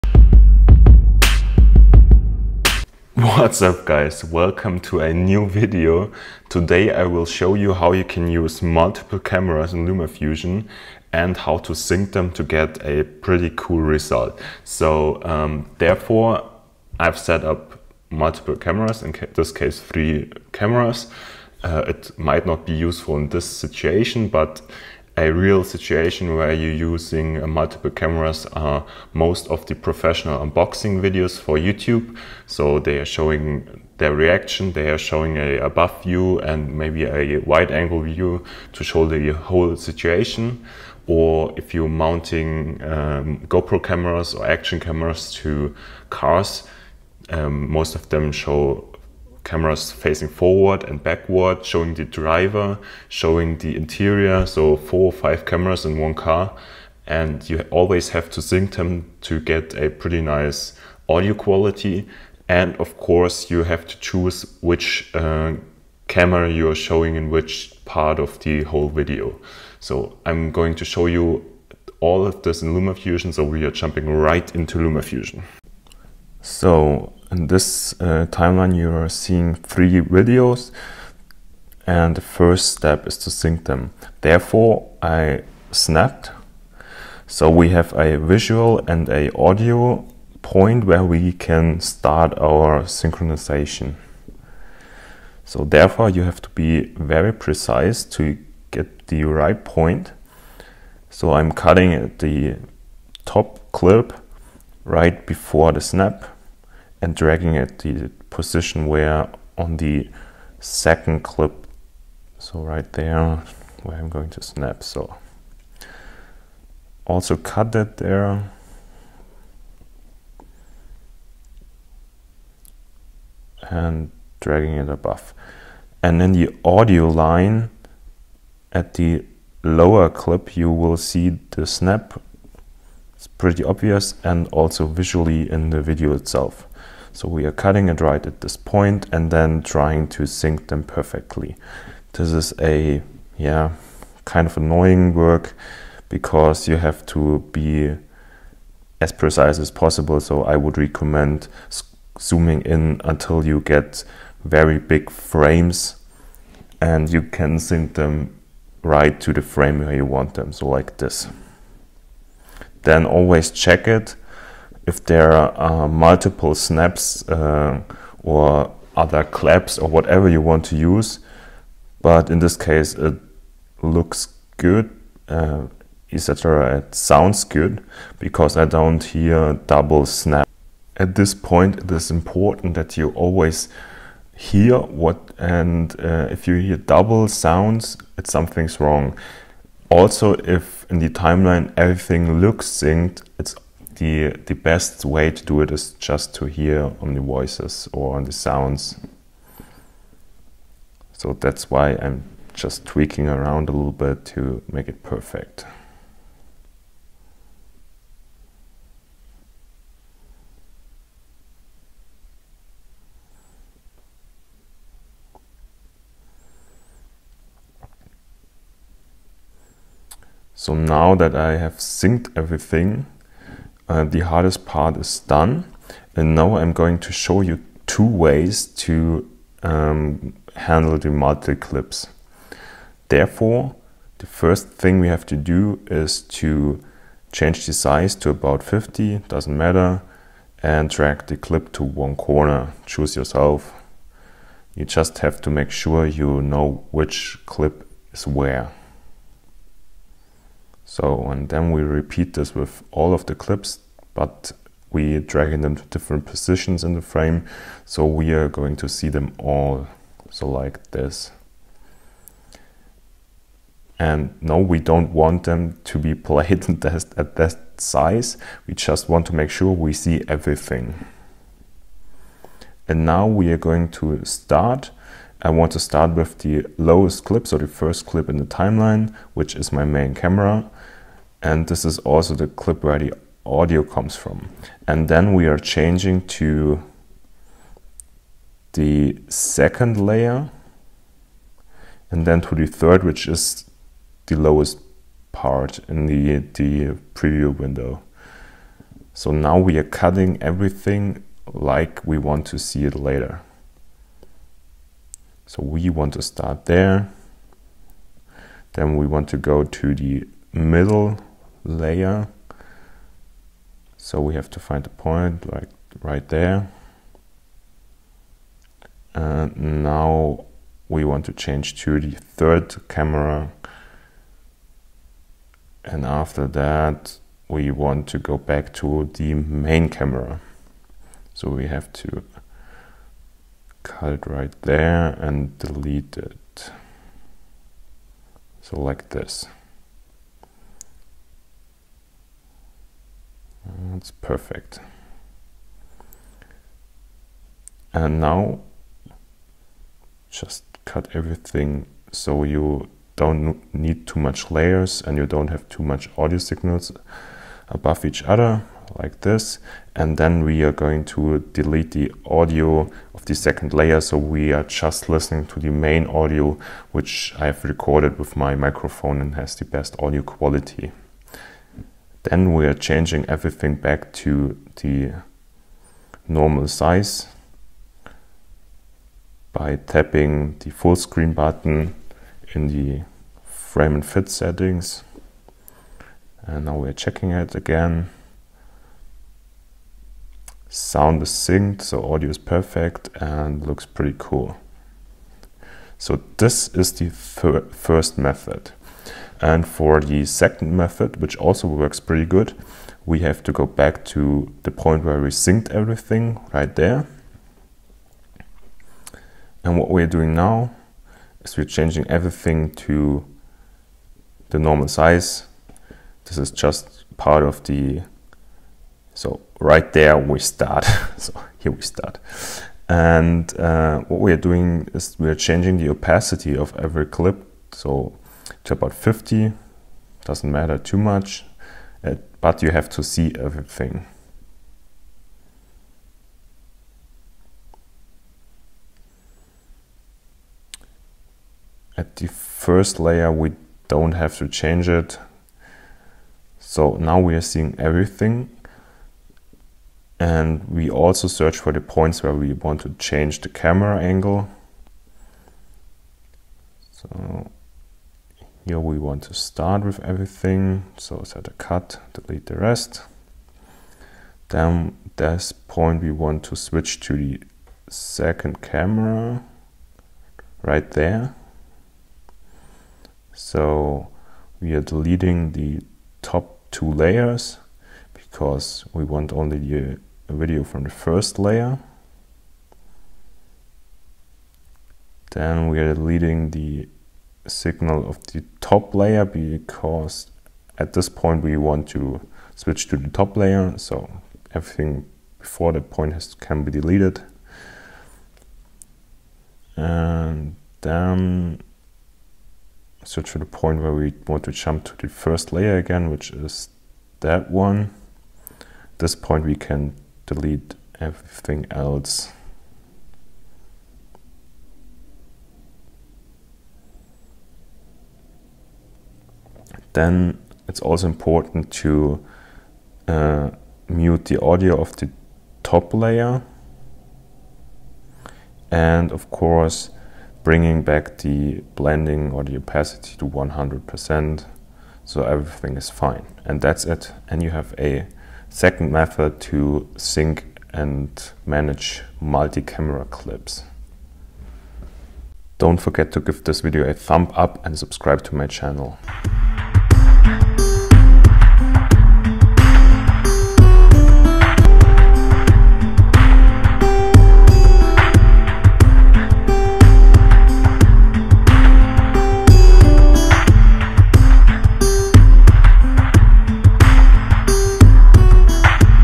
SYNC and CUT MULTIPLE CAMERAS - LumaFusion How to Tutorial
I show you, how to sync multi cameras und how to switch between them in Lumafusion.